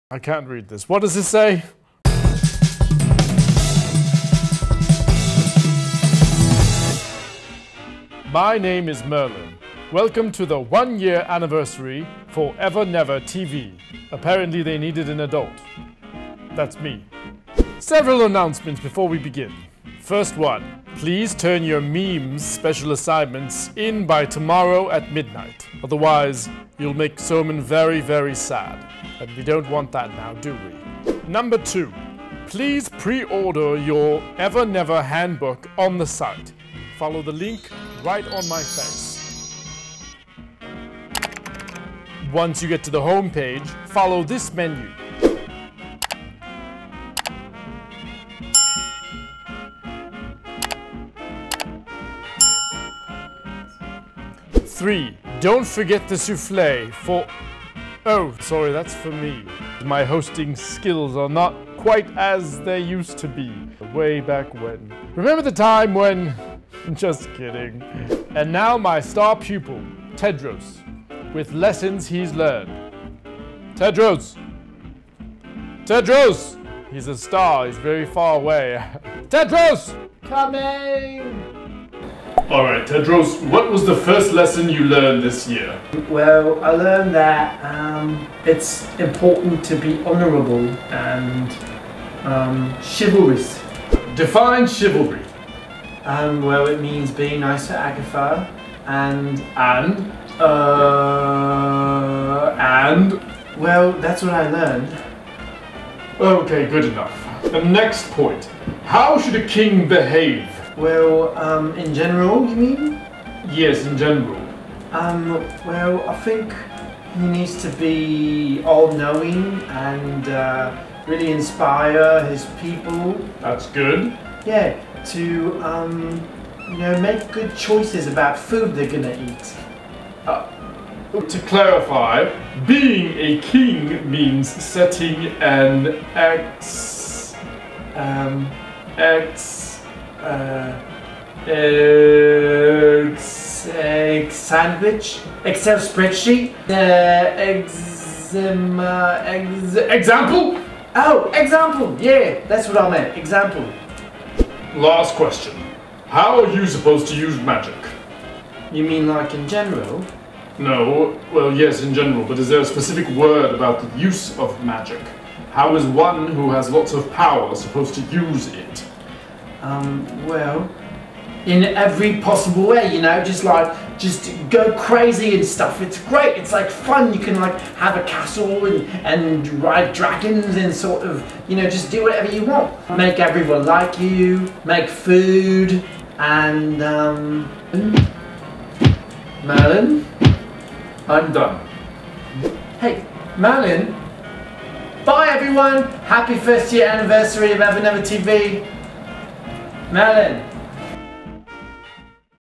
EverNever TV: Introducing Merlin
To celebrate our 1-year anniversary, we've handed the show over to Merlin! Because we needed an adult on the channel.